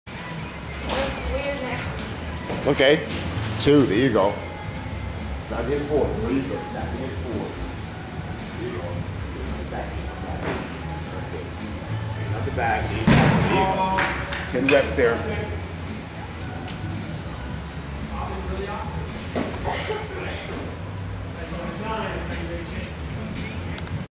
Karsen's softball training videos
Karsen does dead lifts for lower body strength